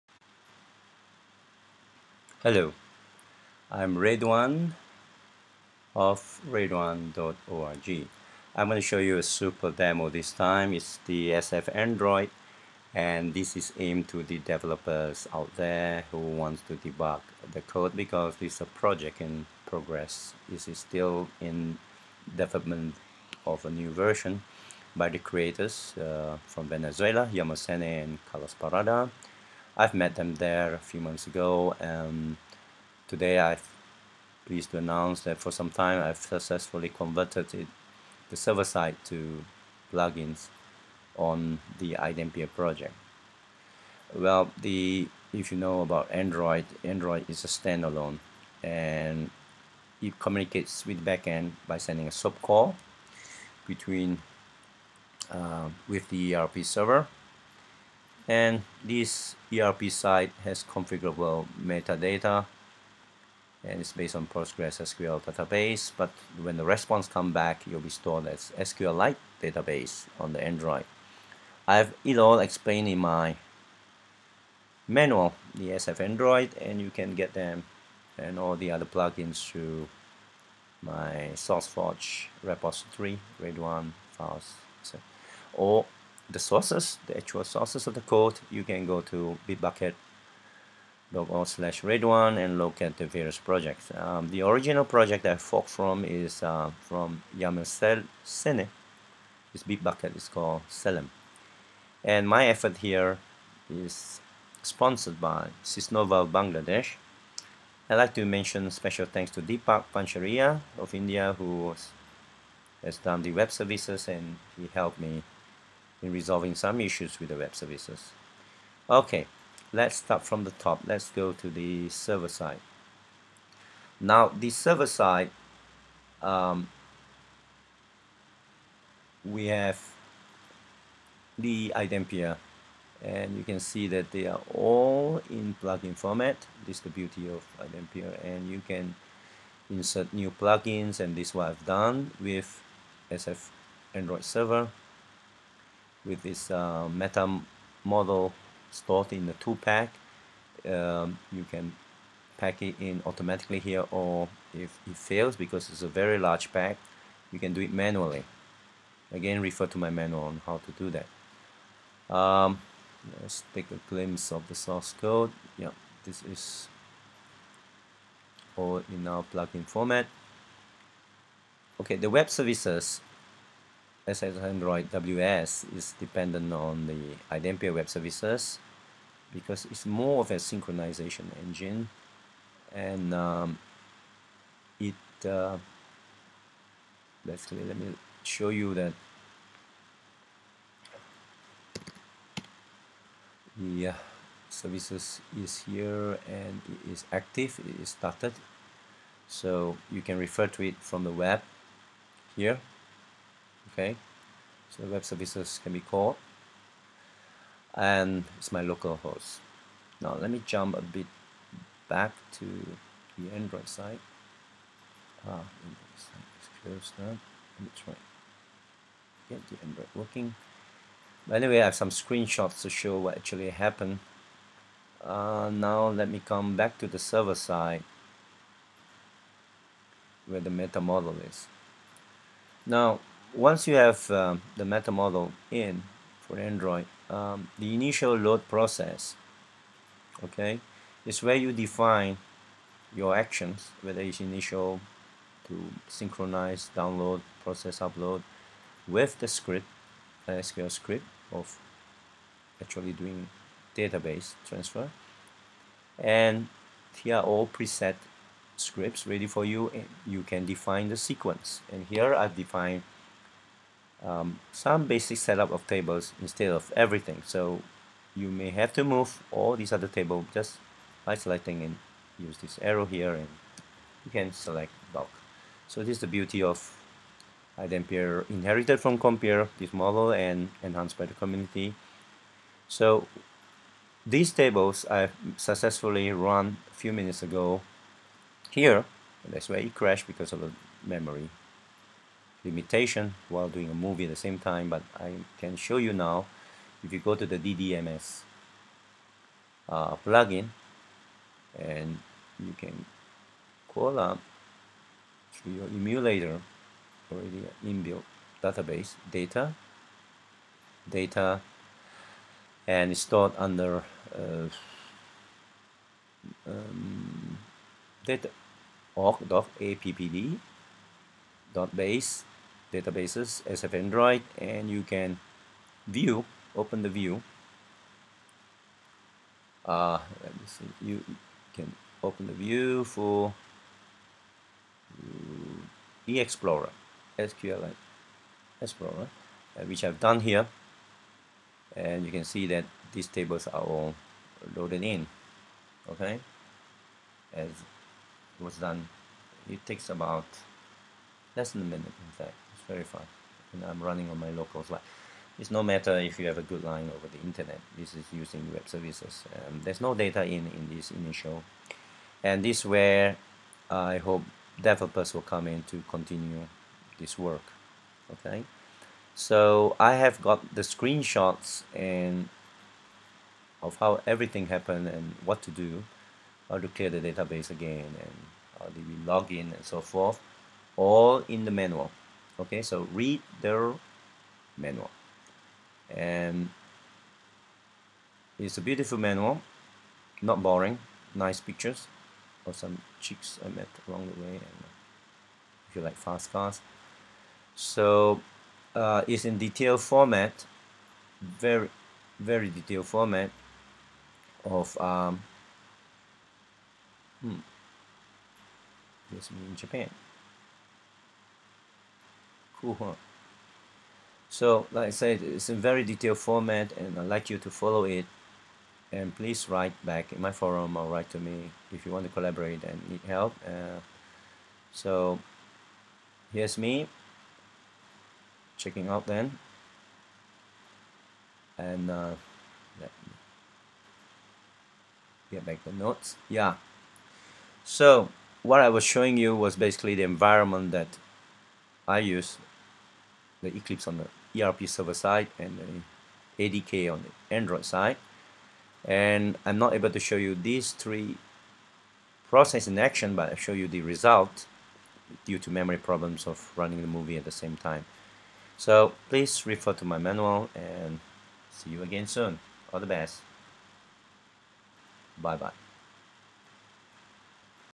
Android ERP Developer Environment
This movie is to further illustrate how the developer environment looks like for both Android and server sides. The SFAndroid project is still in new version development, so i am just showing the successful Initial Load process which sends a SOAP request to the server side via web-services and obtain a response which is stored successfully in the Android mobile unit. My MacBook crashes the ADK everytime i launch the QuickTime screen recorder so i have to record only after successfully running the demo. Thus i use some screen-shots if it.
The meta-data transported via the Web-Service from the ERP side is easily configurable.
This project is the creation of Yamel Senih and Carlos Parada of Venezuela.